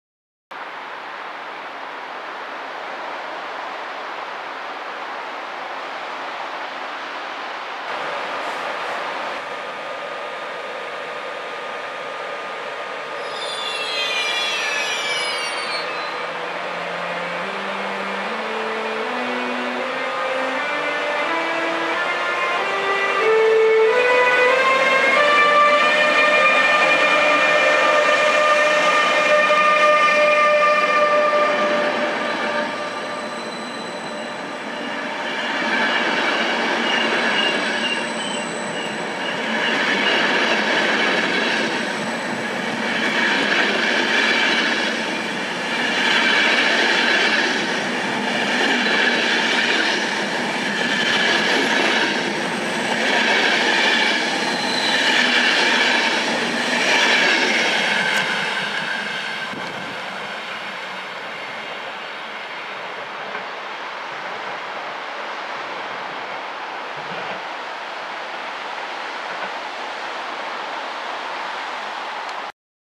ÖBB Taurus loc (1116 155-1) met IC rijtuigen te Bischofshofen
Tijdens de tweede dag van onze 5-daagse Interrailvakantie reisden we van Frankfurt in Duitsland naar Mürzhofen in Oostenrijk. We stapten onder andere over in het Oostenrijkse Bischofshofen, waarbij ik het vertrek van onze EuroCity 113 even op film heb gezet. Voorop een ÖBB Taurus loc met nummer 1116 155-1 met zijn bijzondere optrekgeluid.

2 januari 2013